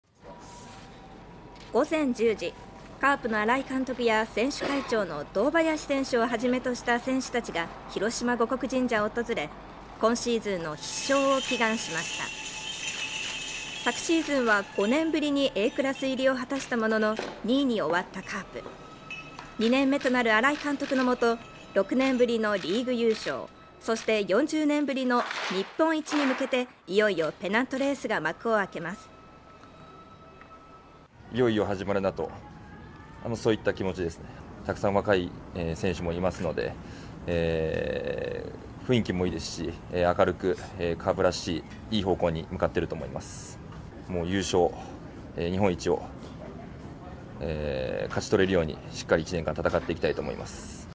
カープ必勝祈願 堂林選手会長「優勝、日本一勝ち取れるようしっかり戦う」 プロ野球２９日開幕 【球団認定】カープ全力応援チャンネル 【球団認定】カープ全力応援チャンネル
プロ野球がいよいよ２９日開幕します。
カープは開幕を前にけさ広島護国神社で必勝祈願を行いました。

午前１０時。カープの新井監督や、選手会長の堂林選手をはじめとした選手たちが広島護国神社を訪れ、今シーズンの必勝を祈願しました。

昨シーズンは５年ぶりにＡクラス入りを果たしたものの２位に終わったカープ。
２年目となる新井監督のもと、６年ぶりのリーグ優勝、そして４０年ぶりの日本一に向けていよいよペナントレースが幕を開けます。

【選手会長・堂林選手】
「いよいよ始まるなという気持ちです。たくさん若い選手もいるので雰囲気もいいし、明るくカープらしいいい方向に向かっている。優勝、日本一を勝ち取れるようにしっかり１年間戦っていきたい」
＊＊＊＊＊＊＊＊＊＊＊＊＊＊＊＊＊＊＊＊＊＊＊＊＊＊＊＊＊＊＊＊＊＊＊＊＊テレビ新広島がオススメするカープの最新情報をお届けするチャンネルです。